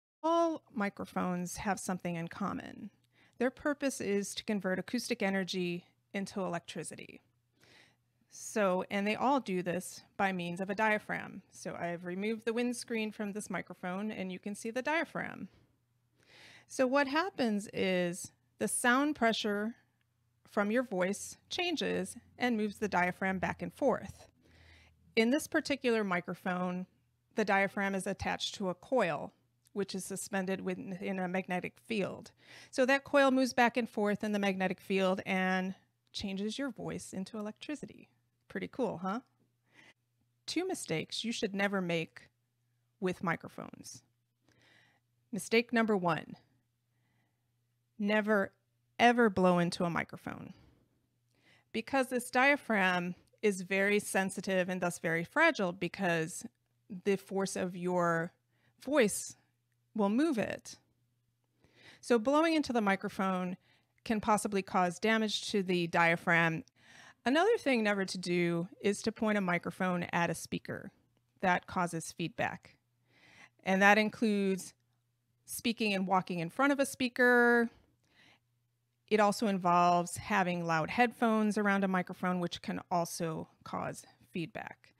Avoid 2 microphone mistakes: Part 2 of 3-part tutorial Microphone Technique for Public Speaking
Grammy-nominated recording engineer Mary Mazurek reveals two common microphone mistakes and how to avoid them, with a demo to explain the science behind it, all in 90 seconds!